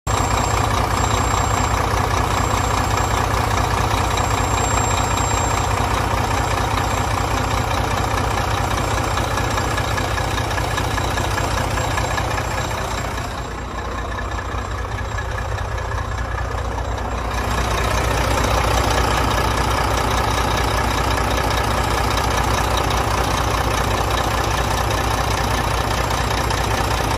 2009 MACK GRANITE GU713 For Sale
2009 Mack Granite GU713 Tri-Axle Dump Truck 405 HP, Mack MP7 Engine, Maxitorque 10 Spd, Camelback Suspension, Ox Bodies Demolition Steel Bed, Front Aluminum and Real Steel Wheels